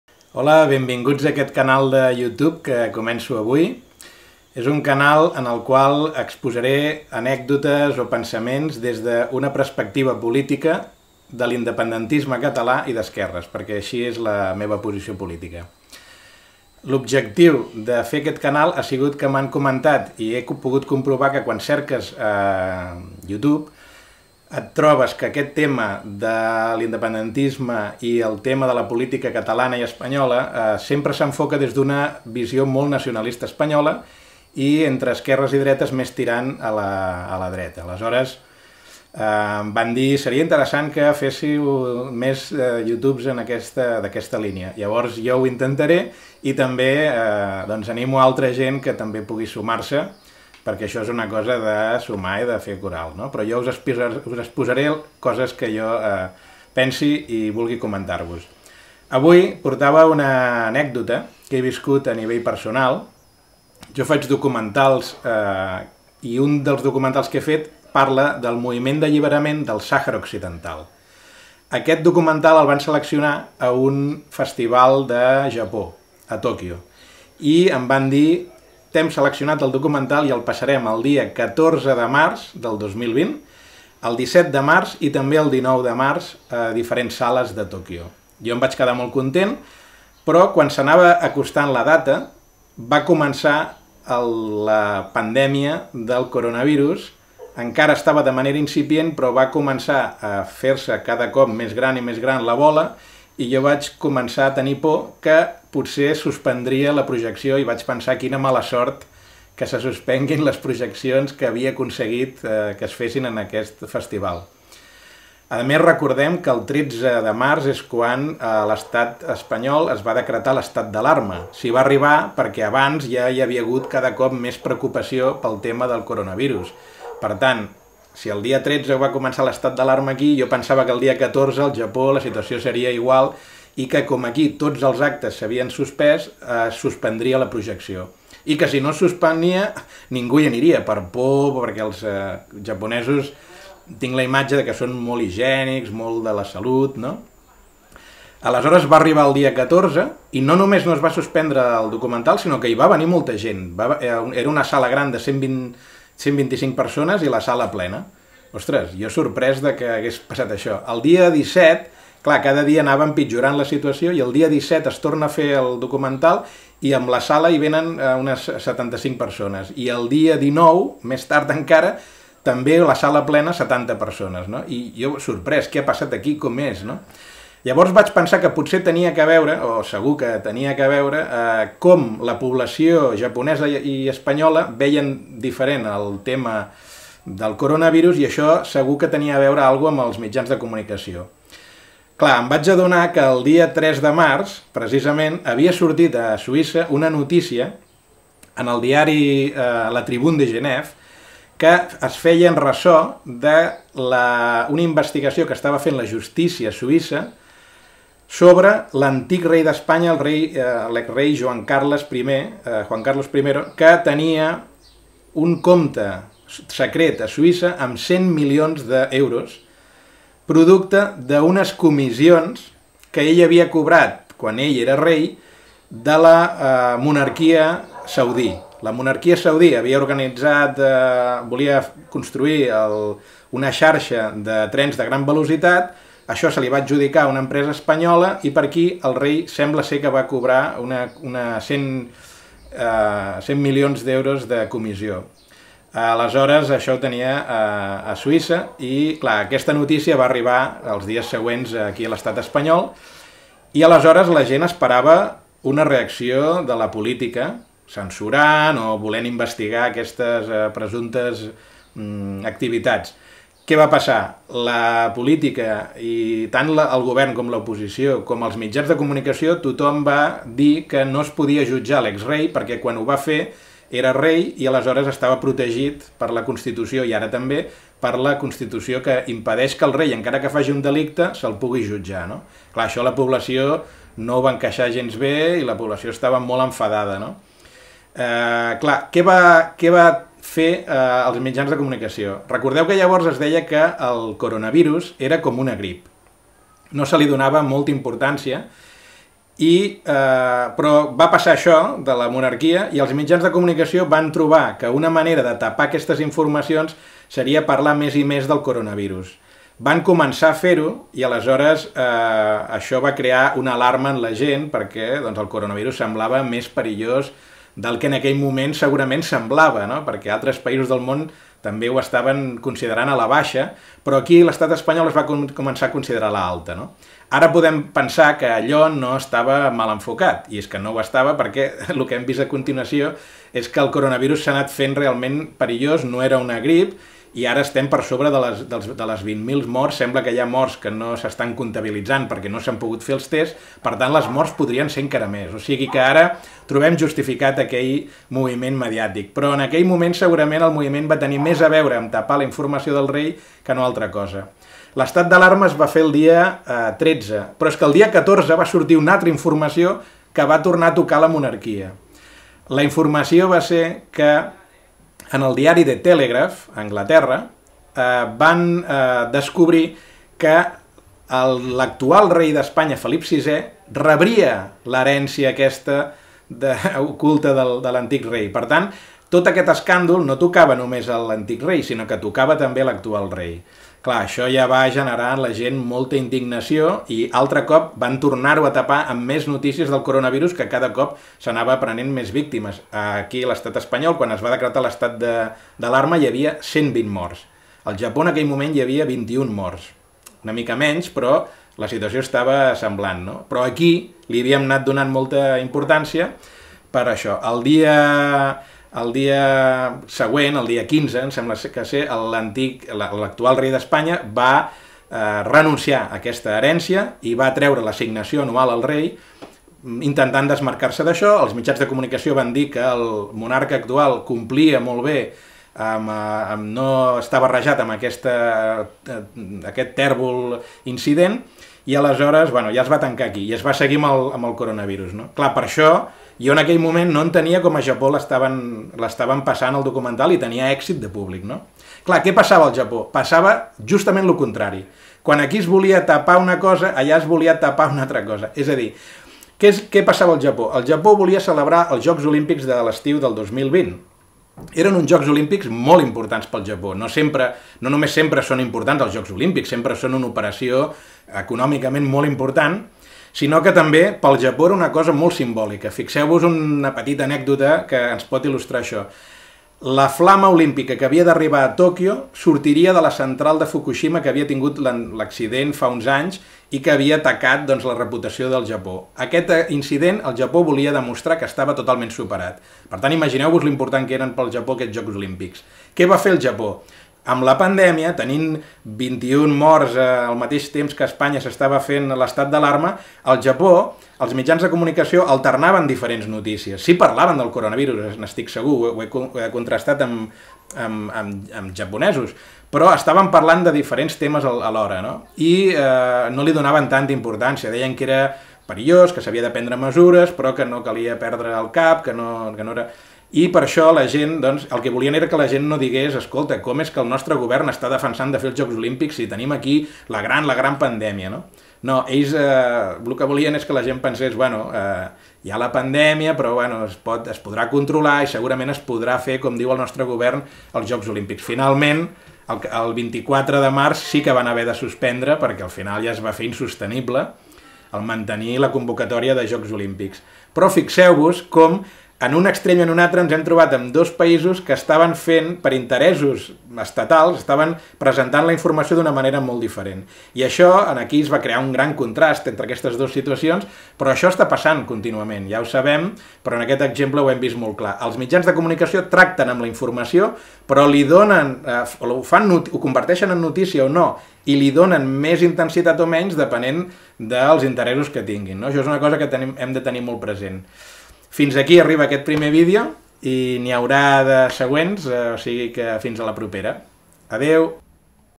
Coronavirus i mass media
Video 1 del canal Calidoscòpic:
Una anècdota il·lustrativa sobre els mitjans de comunicació en temps de coronavirus.